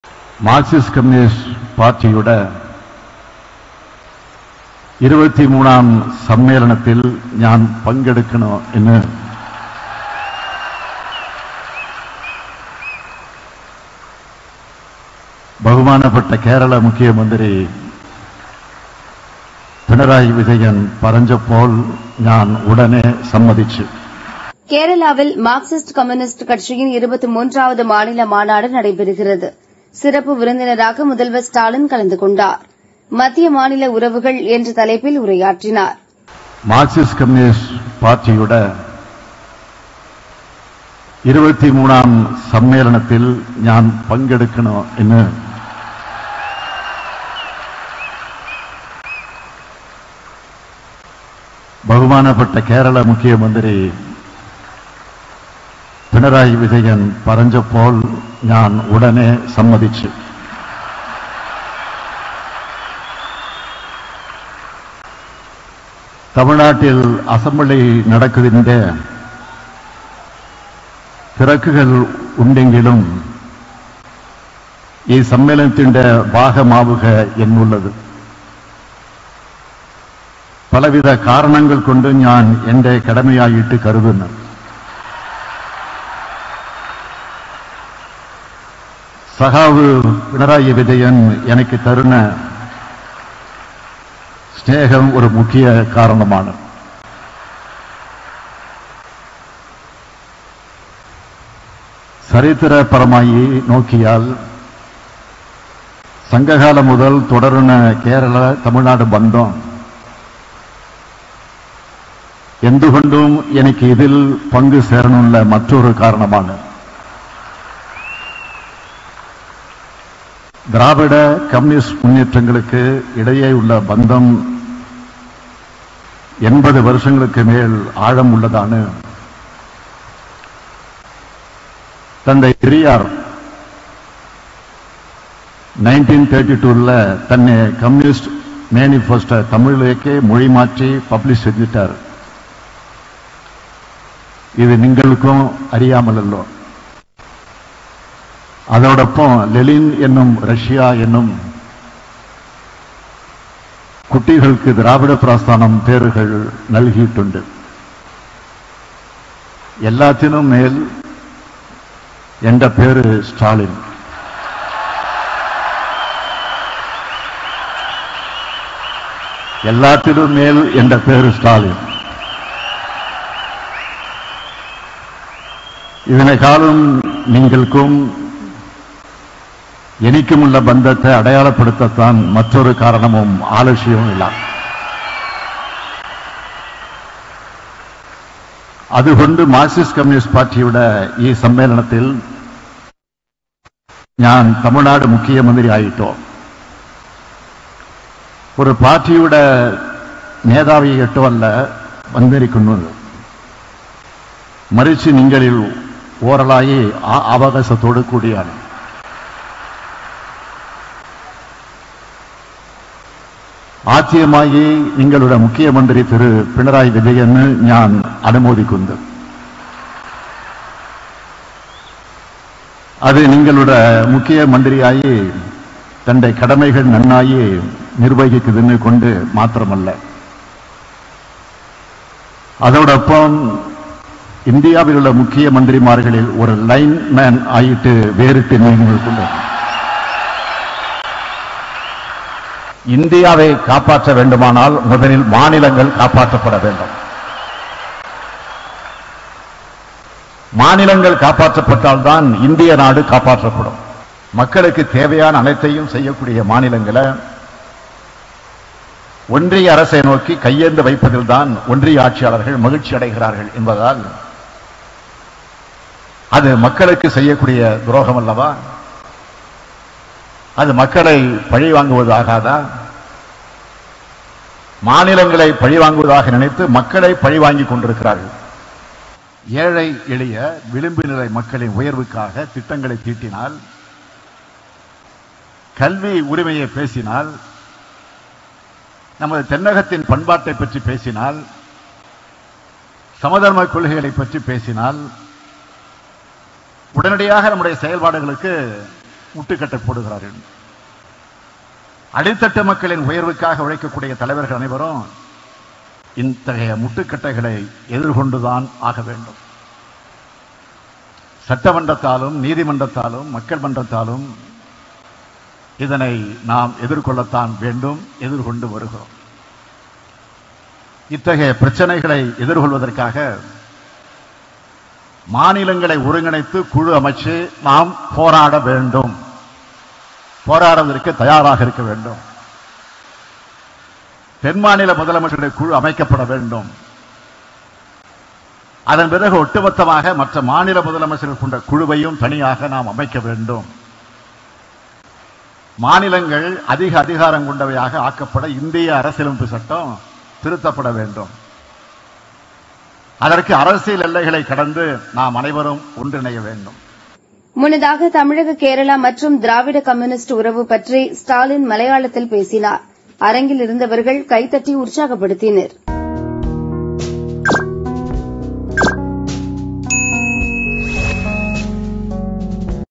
ஸ்டாலின் மலையாள பேச்சு
கேரளாவில், மார்க்சிஸ்ட் கம்யூனிஸ்ட் கட்சியின் 23வது மாநில மாநாடு நடைபெறுகிறது. சிறப்பு விருந்தினராக முதல்வர் ஸ்டாலின் கலந்து கொண்டார்.  &#39;மத்திய, மாநில உறவுகள்&#39; என்ற தலைப்பில் உரையாற்றினார்
முன்னதாக, தமிழக - கேரளா மற்றும் திராவிட- கம்யூனிஸ்ட் உறவு பற்றி ஸ்டாலின் மலையாளத்தில் பேசினார்.